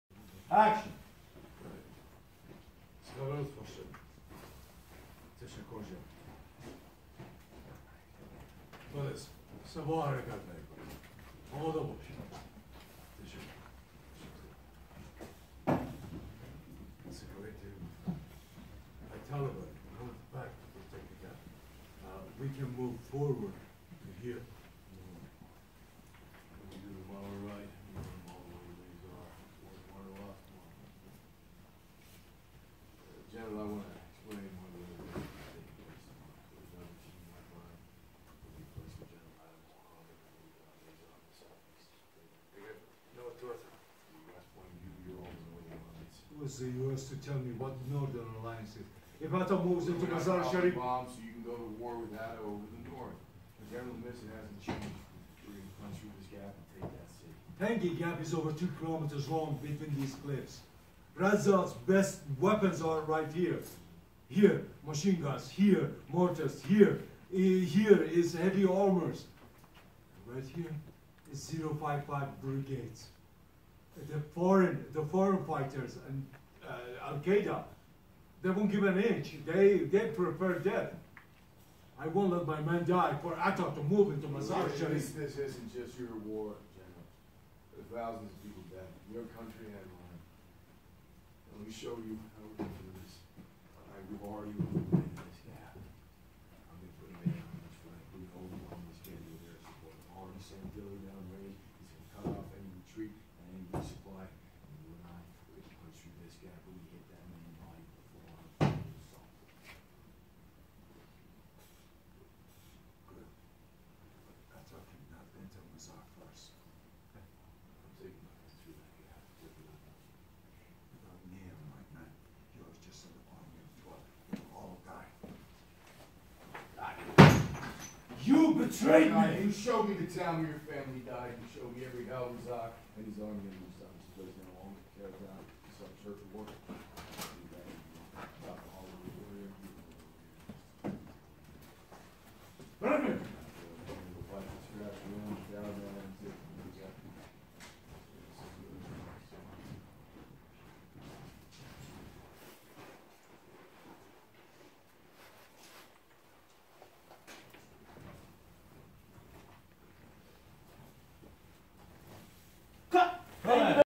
Dolly Grip Crane Tracking Shot Chris Hemsworth 12 Strong
This shot required the construction of a platform inside a cave at White Sands Missile Range - capable of supporting the crane and the Dolly Grip pushing. There is also another Dolly Grip at the lens basically "flying nap of the Earth" with his two hands on the camera, keeping it inches from the jagged rock wall of the cave, stepping on rocks himself and maintaining a steady shot of the two actors.

Two takes.

Level of difficulty 1-10:

This is a 12.